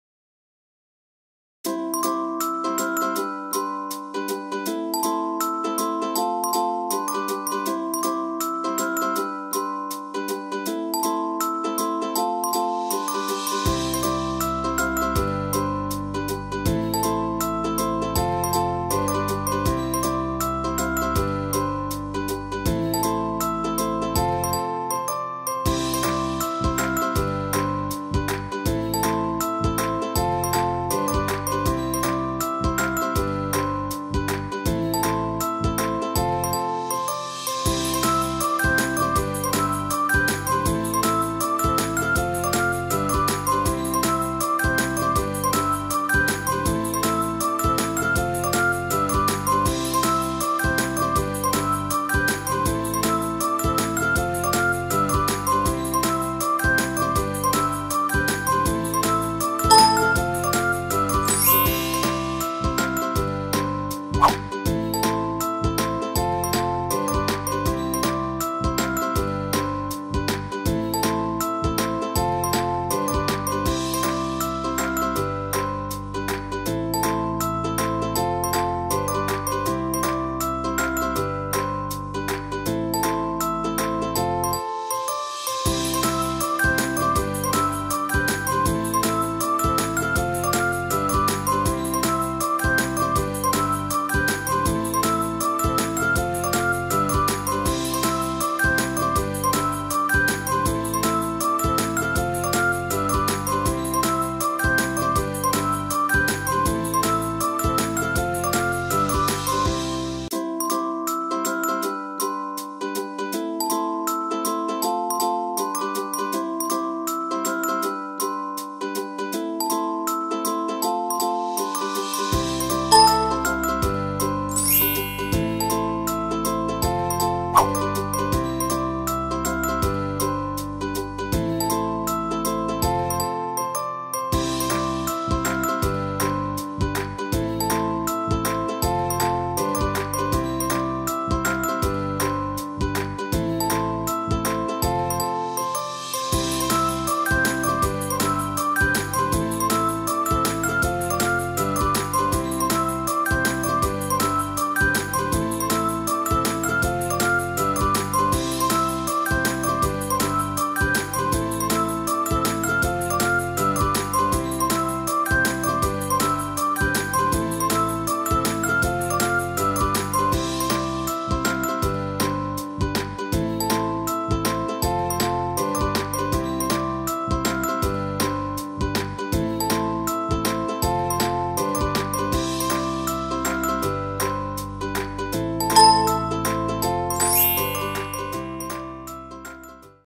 Can you find the 3 differences in 60 seconds?
"Find 3 Differences in 60 Seconds" is a fast-paced visual challenge game consisting of three rounds.

Players are presented with two nearly identical images side by side and must quickly identify three subtle differences between them within a time limit of 60 seconds per round.

Each round offers a new set of images, each with unique variations to detect. With a ticking clock, players must sharpen their observational skills to spot the differences before time runs out.

Ready to test your attention to detail and speed? Play now and see how many differences you can spot in each round!